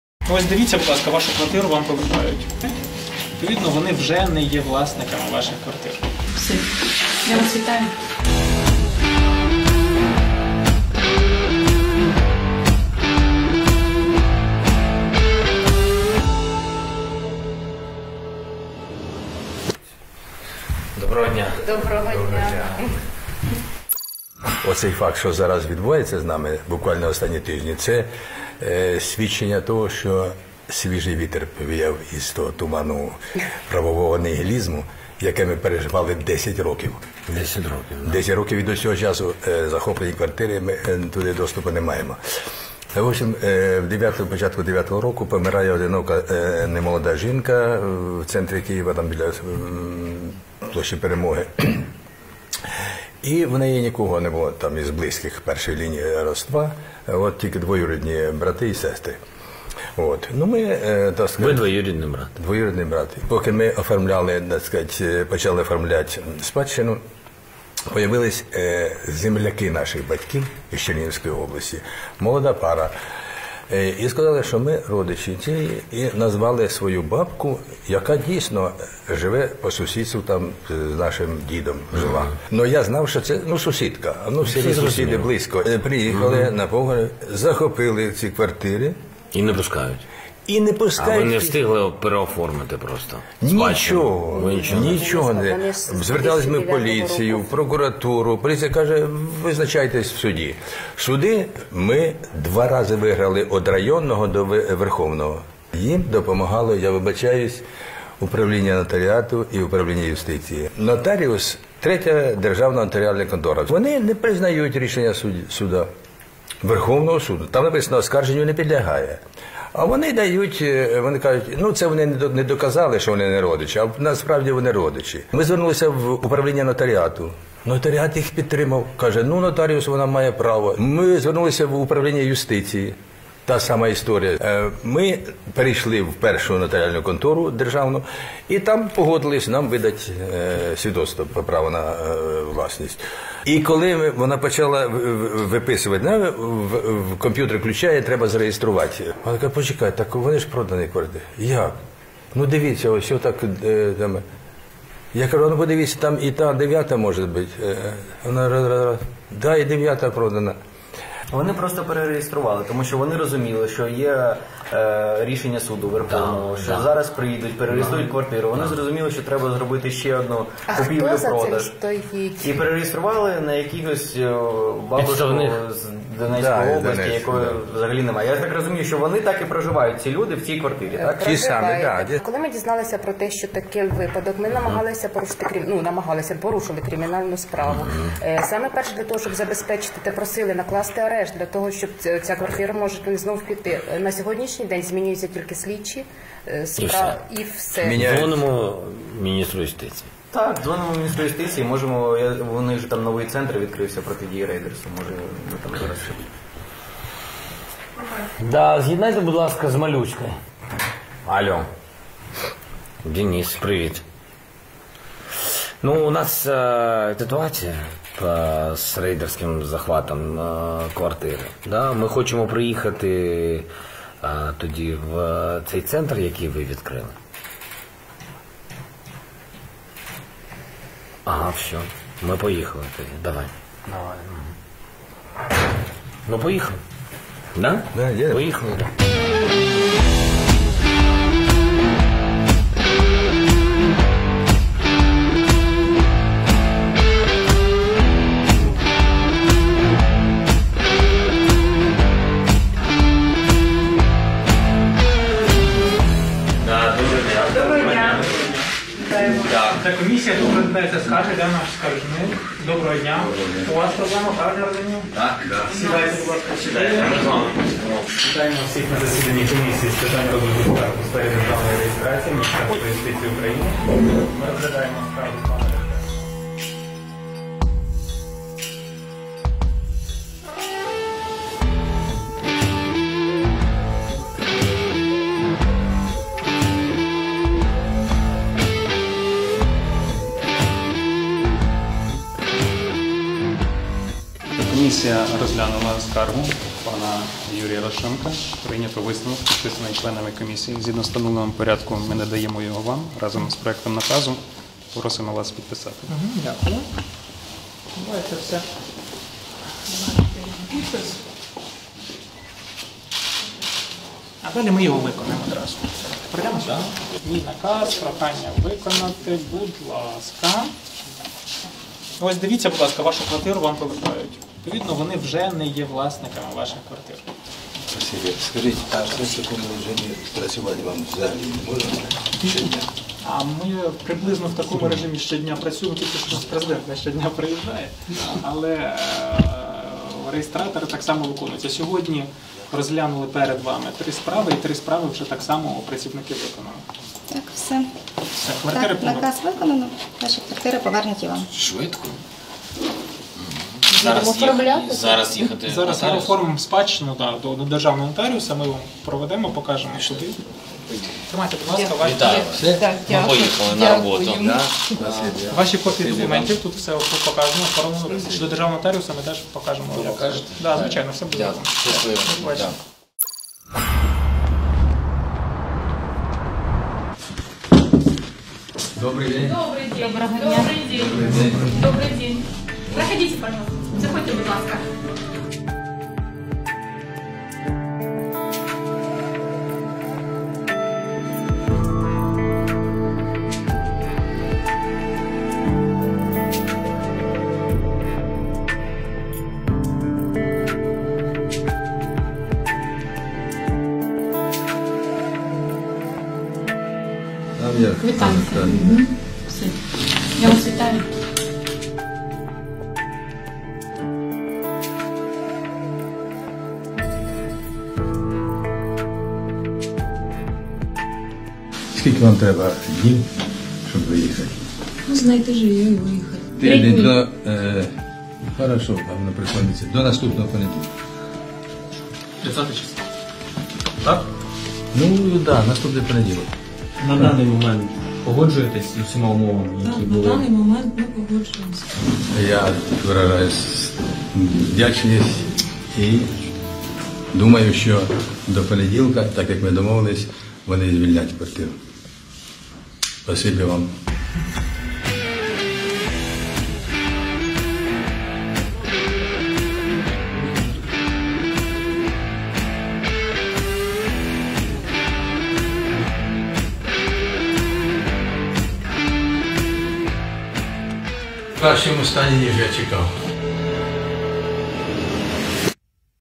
Зеленский показал как работает Офис противодействия рейдерству
В новом видео президент и министр юстиции Малюська возвращают квартиру 81-летнему режиссеру Юрию Ляшенко.